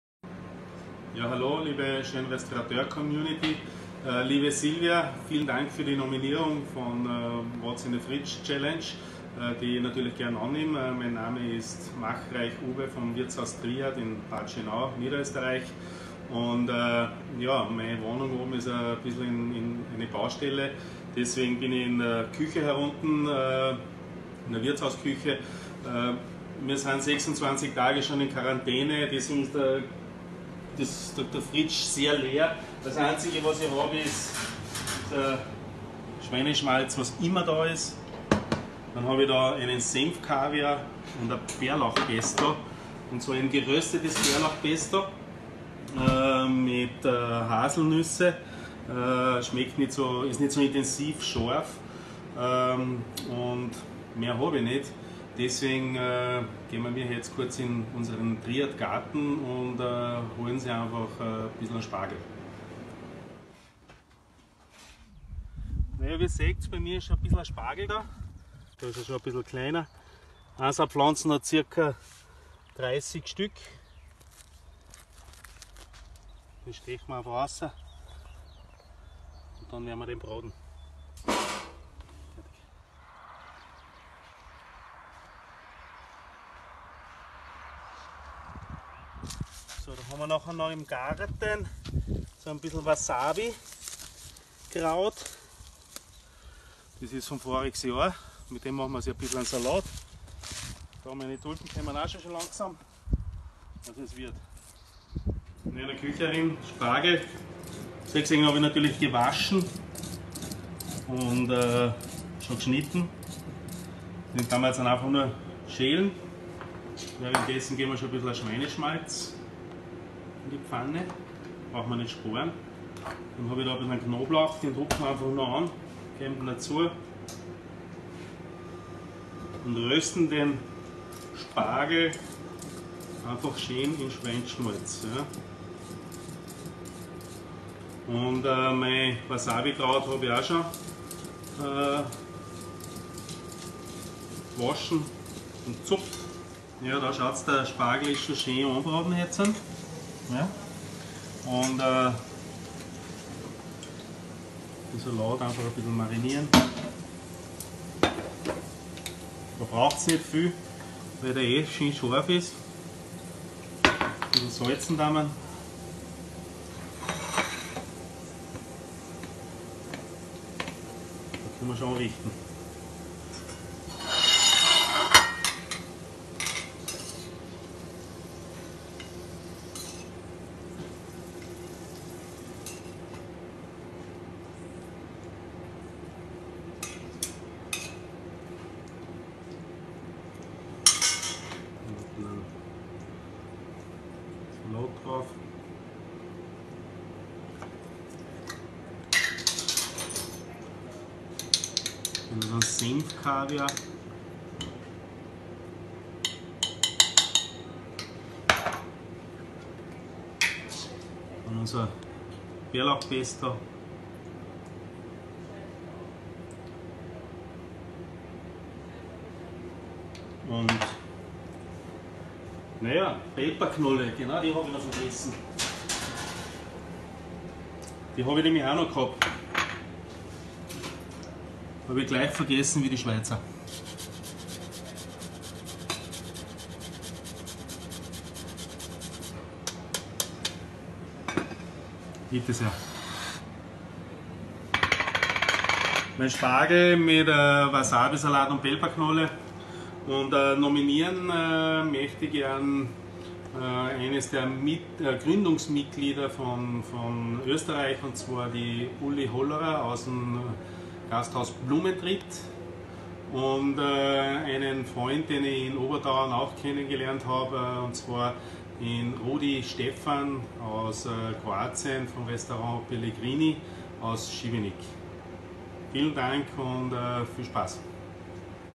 What's in the fridge challenge - Uwe Machreich
WHAT'S IN THE FRIDGE CHALLENGE!

Curious about the fridge/garden of JRE-Österreich chef Uwe Machreich from Triad? Let's have a look.